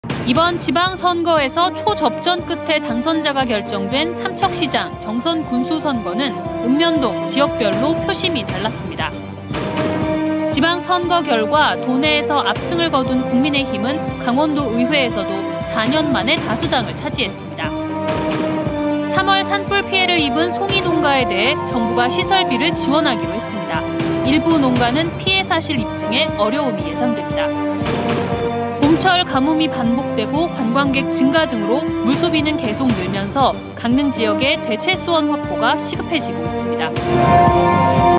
헤드라인 220608
이번 지방선거에서 초접전 끝에 당선자가 결정된 삼척시장, 정선군수 선거는 읍면동, 지역별로 표심이  달랐습니다.

지방선거 결과, 도내에서 압승을 거둔 국민의힘은 강원도의회에서도4년 만에  다수당을 차지했습니다.

3월 산불 피해를 입은 송이 농가에 대해 정부가 시설비를 지원하기로 했습니다. 일부 농가는 피해 사실 입증에 어려움이 예상됩니다.

봄철 가뭄이 반복되고 관광객 증가 등으로 물 소비가 계속 늘면서 강릉 지역의  대체 수원 확보가 시급해지고 있습니다.